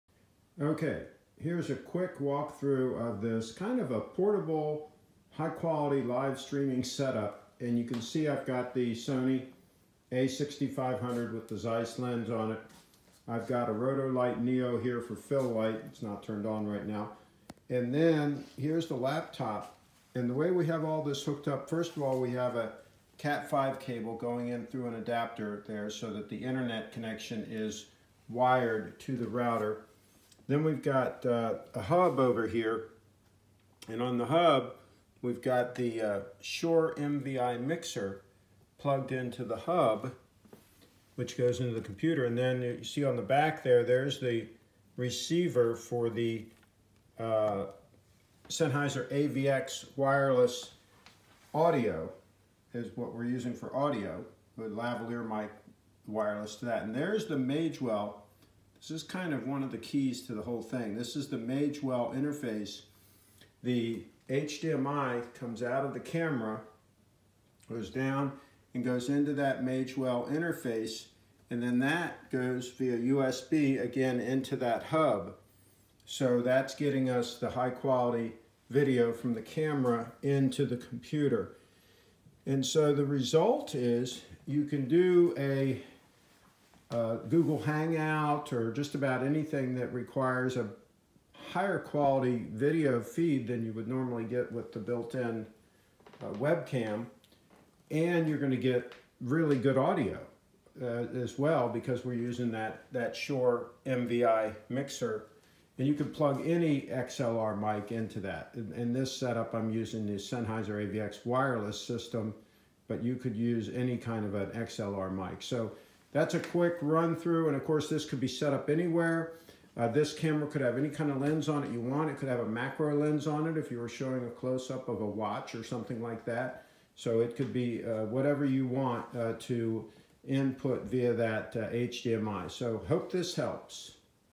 Portable LIVE streaming setup
Here’s a setup that allows you to get high quality video and audio into your laptop for LIVE streaming on YouTube.

In this LIVE streaming setup I’m using a Magewell HDMI to USB interface to get video from my Sony a6500 into my MacBook Pro. For audio my Sennheiser AVX ME2 lav into a Shure MVi audio interface also into the USB hub and into the laptop.